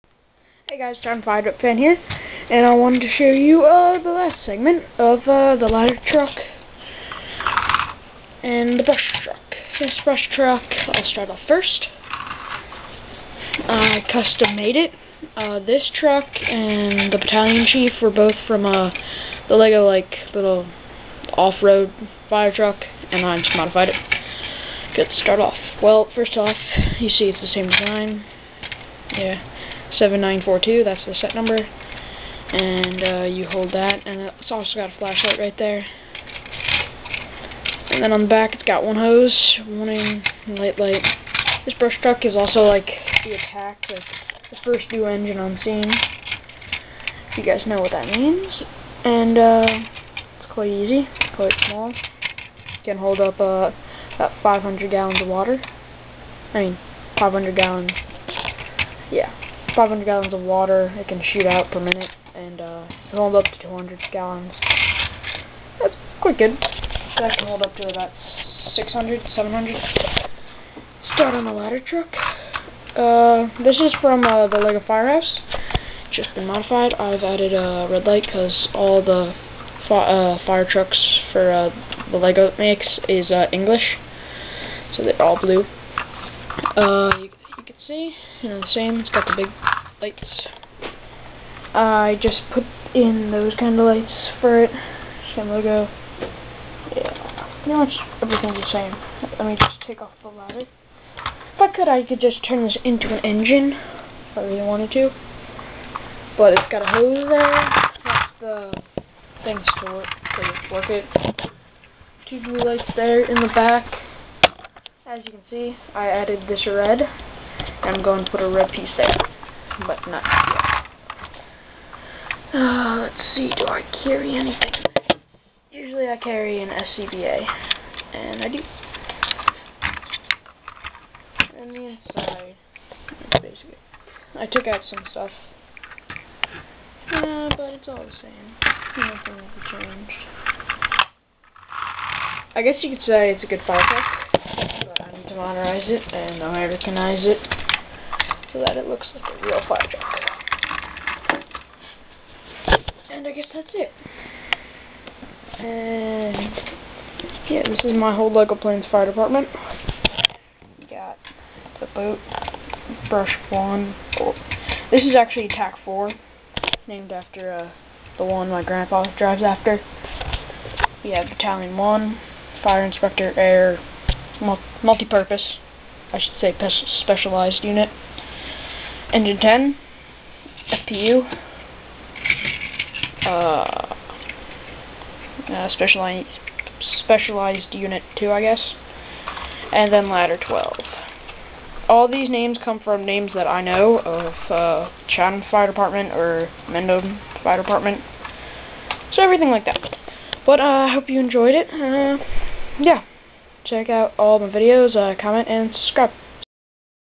Lego fire truck
Lego plain's ladder and brush well the brush is also attack 4 there isn't a forest so its attack 4 name after brookside engine company attack 4